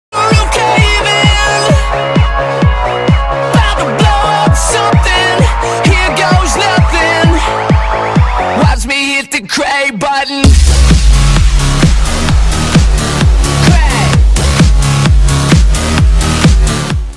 Fly’s first agility sequence (8 months)
Finally we put some things together and it started looking like proper agility training.